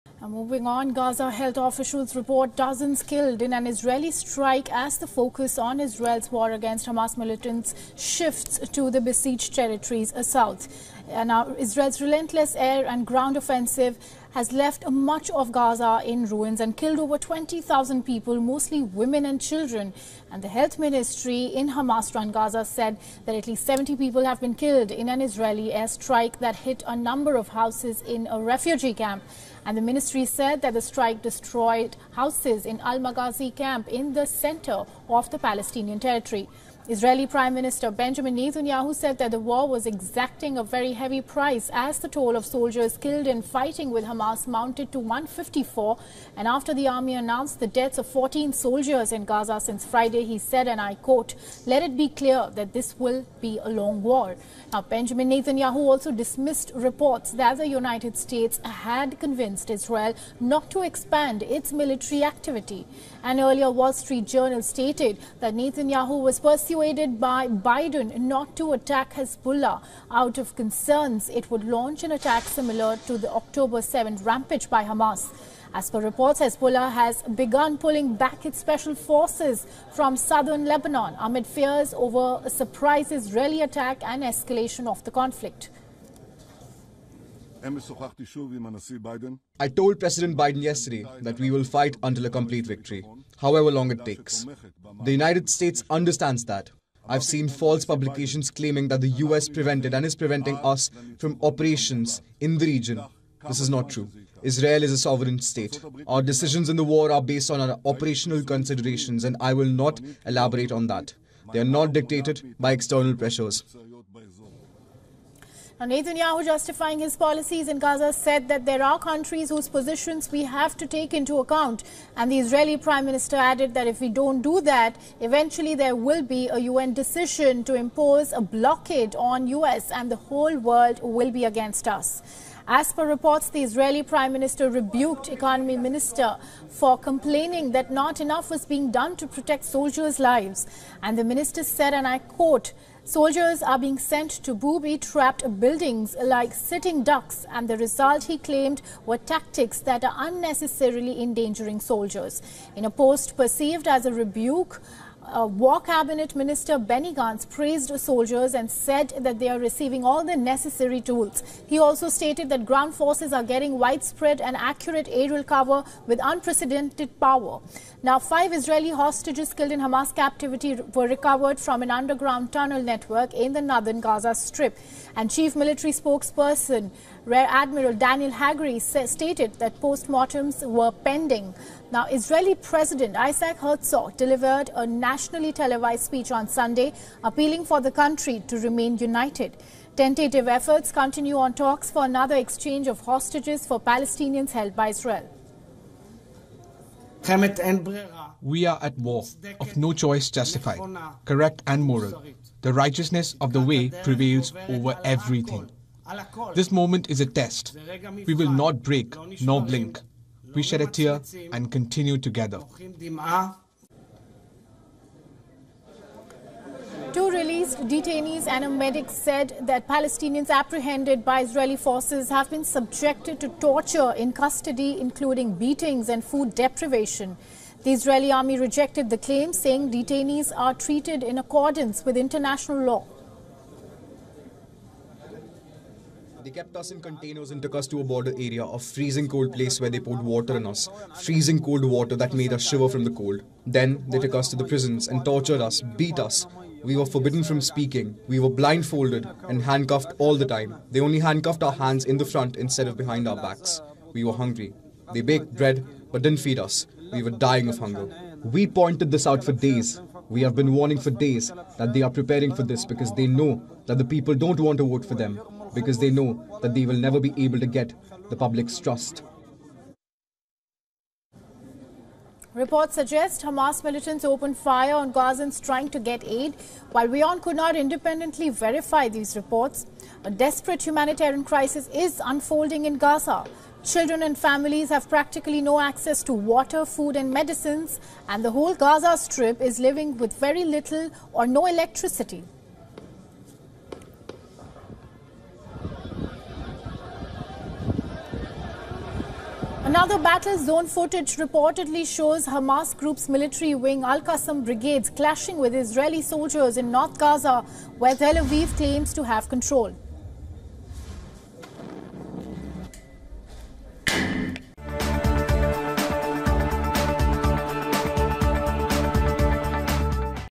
Gaza officials: Homes destroyed in Israeli strikes on Al-Maghazi camp, at least 70 killed | WION
Gaza Health officials report dozens killed in an Israeli strike as the focus on Israel's war against Hamas militants shifts to the besieged territories. Israel's relentless air and ground offensive has left much of Gaza in ruins and killed over 20,000 people mostly women and children.